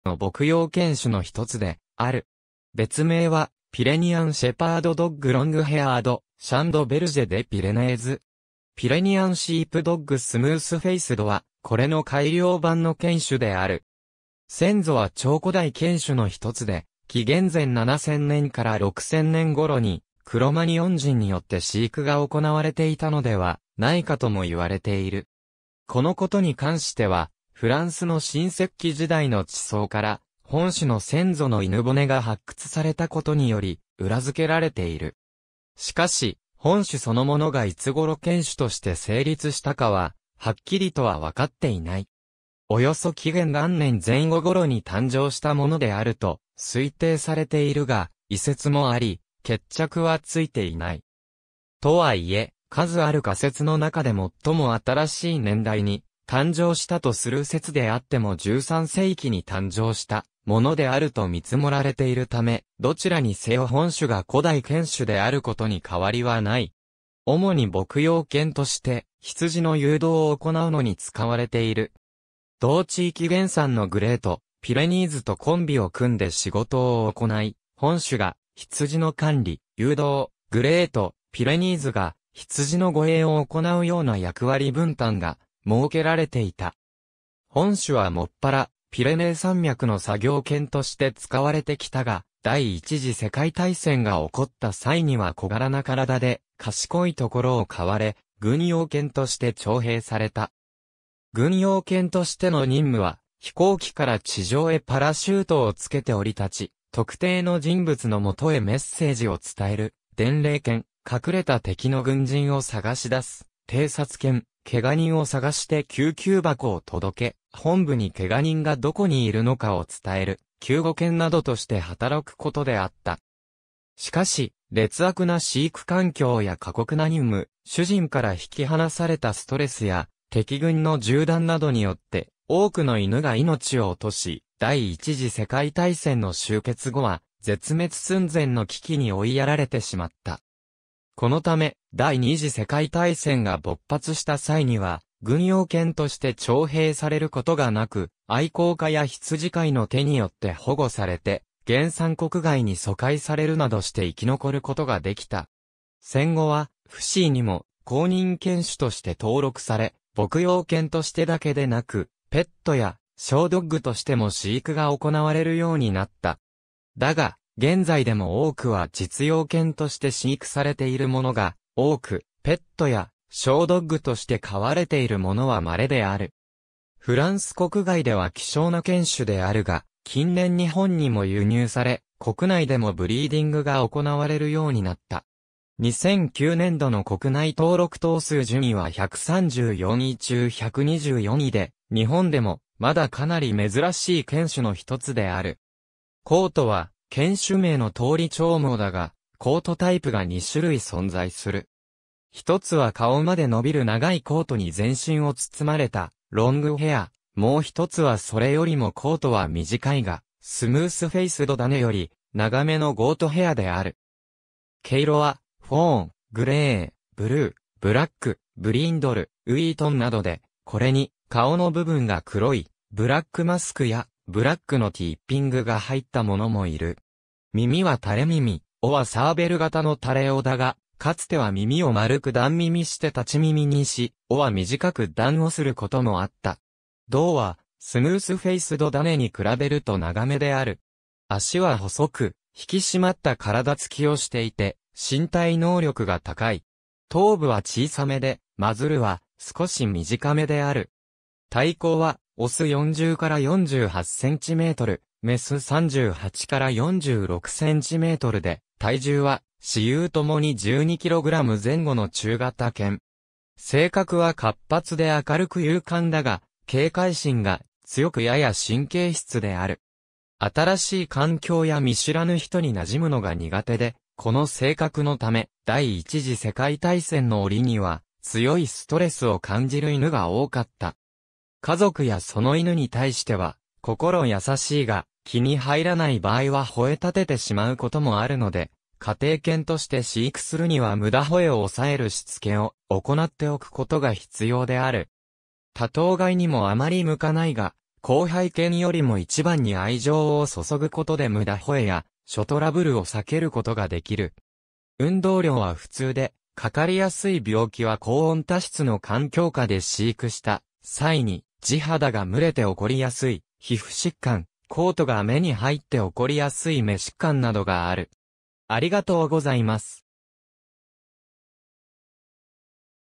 ピレニアン・シープドッグ・ロングヘアード
ピレニアン・シープドッグ・ロングヘアード（英: Pyrenean Sheepdog Long-haired）は、フランスのピレネー山脈原産の牧羊犬種のひとつである。
別名はピレニアン・シェパード・ドッグ・ロングヘアード（英: Pyrenean Shephrd Dog Long-haired）、シャン・ド・ベルジェ・デ・ピレネーズ（英: Chien de Berger des Pyrenese）。
ピレニアン・シープドッグ・スムースフェイスドは、これの改良版の犬種である。
先祖は超古代犬種の一つで、紀元前7000年〜6000年ごろにクロマニヨン人によって飼育が行なわれていたのではないかともいわれている。
このことに関しては、フランスの新石器時代の地層から本種の先祖の犬骨が発掘されたことにより裏付けられている。
しかし、本種そのものがいつごろ犬種として成立したかははっきりとは分かっていない。
およそ紀元元年前後ごろに誕生したものであると推定されているが、異説もあり決着はついていない。
とはいえ、数ある仮説の中でもっとも新しい年代に誕生したとする説であっても13世紀に誕生したものであると見積もられているため、どちらにせよ本種が古代犬種であることに変わりはない。
主に牧羊犬として羊の誘導を行なうのに使われている。
同地域原産のグレート・ピレニーズとコンビを組んで仕事を行い、本種が羊の管理・誘導（牧羊）、グレート・ピレニーズが羊の護衛（護畜）を行うような役割分担が設けられていた。
本種は専らピレネー山脈の作業犬として使われてきたが、第一次世界大戦が起こった際には小柄な体で賢いところを買われ、軍用犬として徴兵された。
軍用犬としての任務は飛行機から地上へパラシュートをつけて降り立ち、特定の人物のもとへメッセージを伝える伝令犬、隠れた敵の軍人を探し出す偵察犬、ケガ人を探して救急箱を届け、本部にケガ人がどこにいるのかを伝える救護犬などとして働くことであった。
然し、劣悪な飼育環境や苛酷な任務、主人から引き離されたストレスや敵軍の銃弾などによって多くの犬が命を落とし、第一次世界大戦の終結後は絶滅寸前の危機に追いやられてしまった。
このため、第二次世界大戦が勃発した際には軍用犬として徴兵されることがなく、愛好家や羊飼いの手によって保護されて原産国外に疎開されるなどして生き残ることが出来た。
戦後はFCIにも公認犬種として登録され、牧羊犬としてだけでなくペットやショードッグとしても飼育が行なわれるようになった。
だが、現在でも多くは実用犬として飼育されているものが多く、ペットやショードッグとして飼われているものは稀である。
フランス国外では希少な犬種であるが、近年日本にも輸入され、国内でもブリーディングが行なわれるようになった。
2009年度の国内登録頭数順位は134位中124位で、日本でもまだかなり珍しい犬種の一つである。
コートは犬種名の通り長毛だが、コートタイプが2種類存在する。
1つは顔まで伸びる長いコートに全身を包まれたロングヘア、もう1つはそれよりもコートは短いが、スムースフェイスド種より長めのゴートヘアである。
毛色はフォーン、グレー、ブルー、ブラック、ブリンドル、ウイートン（小麦色）などで、これに顔の部分（耳やマズルなど）が黒い「ブラックマスク」や、ブラックのティッピングが入ったものもいる。
耳は垂れ耳、尾はサーベル形の垂れ尾だが、かつては耳を丸く断耳して立ち耳にし、尾は短く断尾することもあった。
胴はスムースフェイスド種に比べると長めである。
脚は細く、引き締まった体つきをしていて、身体能力が高い。
頭部は小さめで、マズルは少し短めである。
体高は雄40〜48cm、雌38〜46cmで、体重は雌雄共に12kg前後の中型犬。
性格は活発で明るく勇敢だが、警戒心が強くやや神経質である。
新しい環境や見知らぬ人になじむのが苦手で、この性格のため第一次世界大戦の折には強いストレスを感じる犬が多かった。
家族やその犬に対しては心優しいが、気に入らない場合は吠え立ててしまうこともあるので、家庭犬として飼育するには無駄吠えを抑えるしつけを行なっておくことが必要である。
多頭飼いにもあまり向かないが、後輩犬よりも一番に愛情を注ぐことで無駄吠えや諸トラブルを避けることが出来る。
運動量は普通で、かかりやすい病気は高温多湿の環境下で飼育した際に地肌が蒸れて起こりやすい皮膚疾患、コートが目に入って起こりやすい眼疾患などがある。